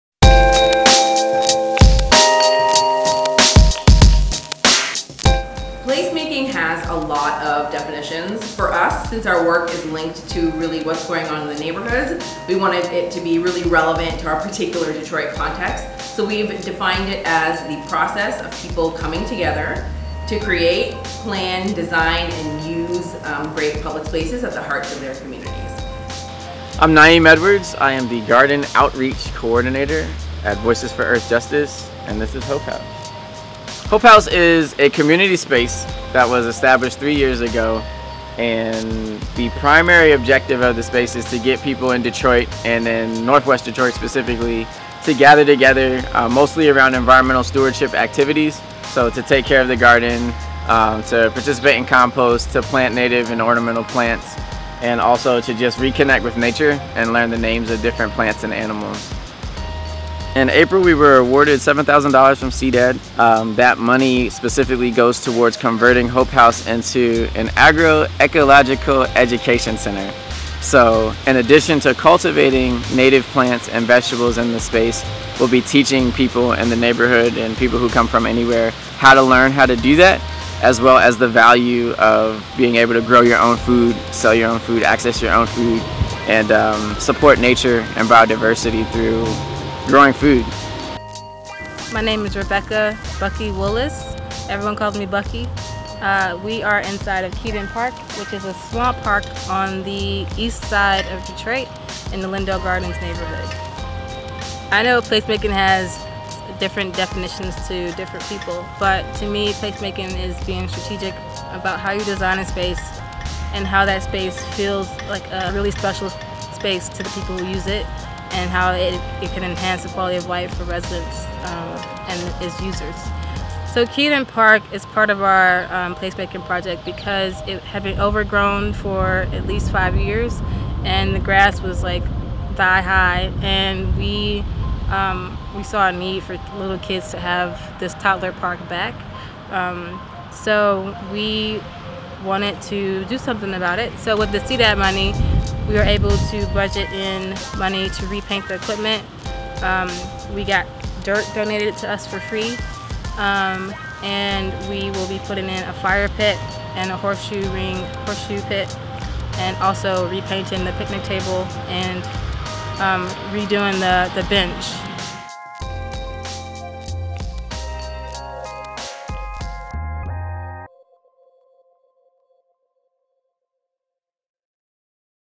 Placemaking in Detroit
In the summer of 2015 CDAD awarded seven placemaking projects to Detroit neighborhoods. This video highlights a few of those projects and the importance of neighborhood placemaking.